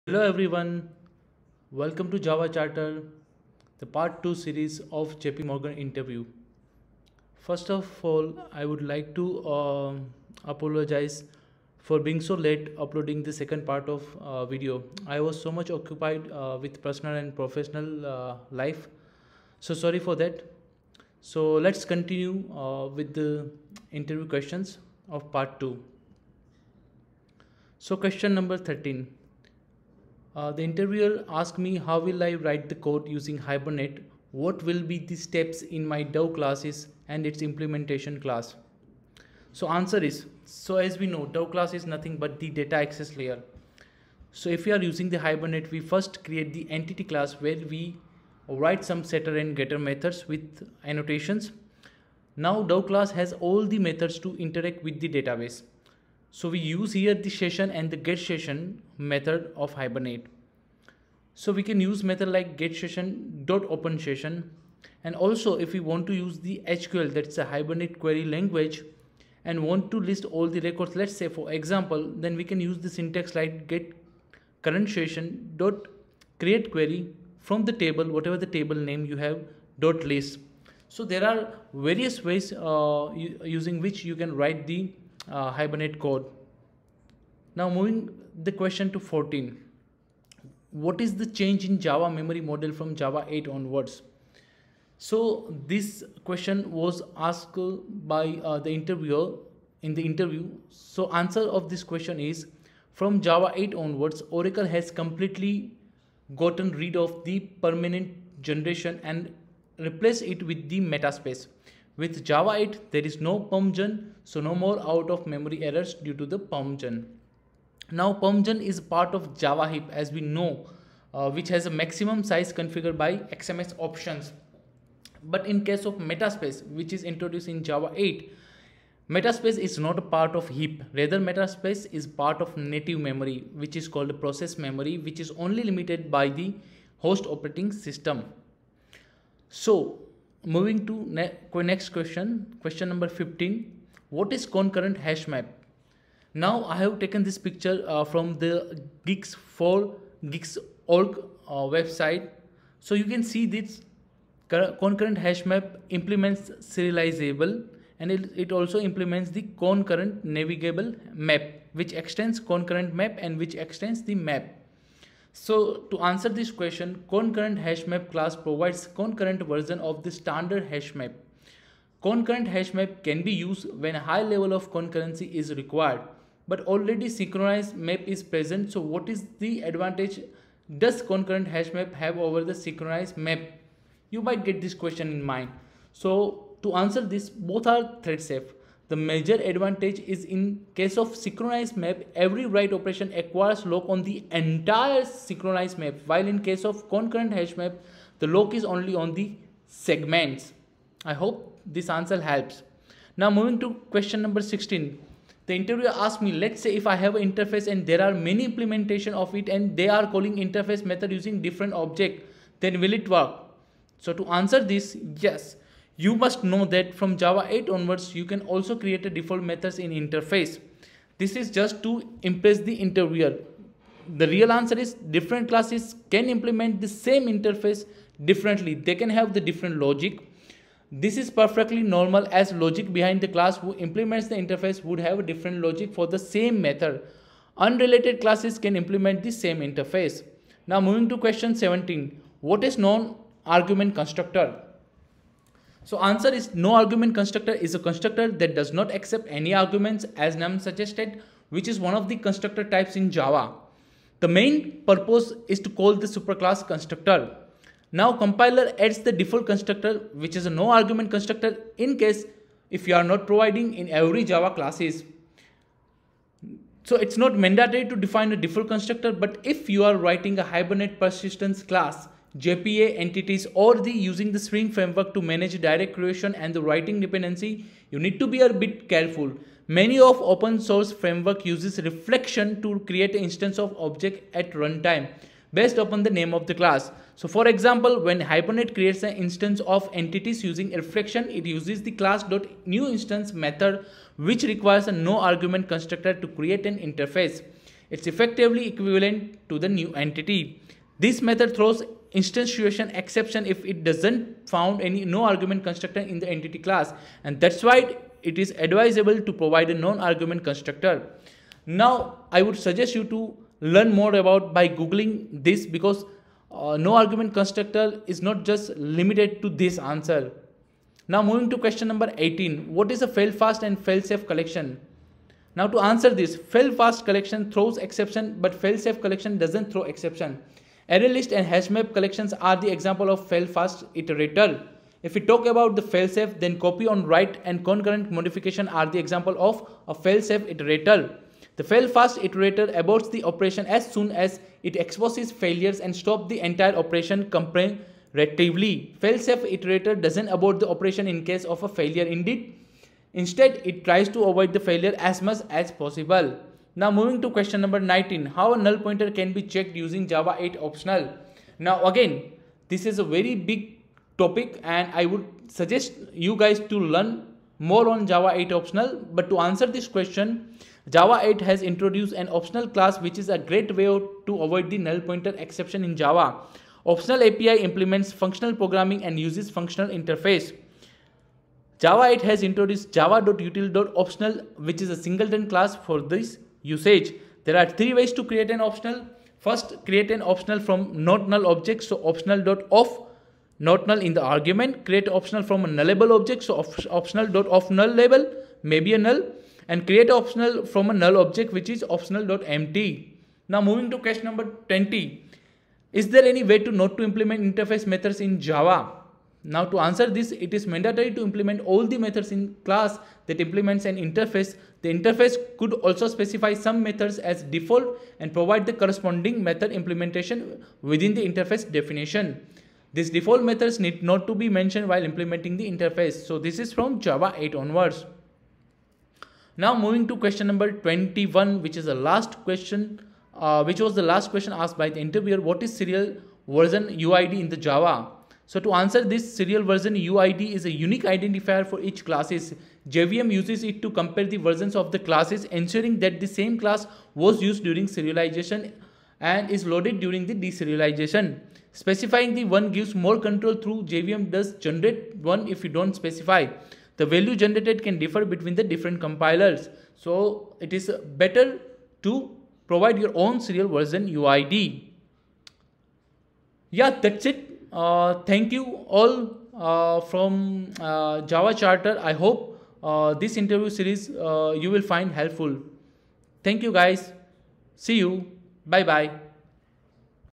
JPMorgan Java interview experience (Part-II) | Interview questions and answers | 10 + years
Hello Coders and Programmers,

Here I'm sharing part-II of my interview experience with JP Morgana. Sorry for late uploading.

This video aims to help job seekers.

jp morgan Interview Question and Answers

Thanks,
JavaCharter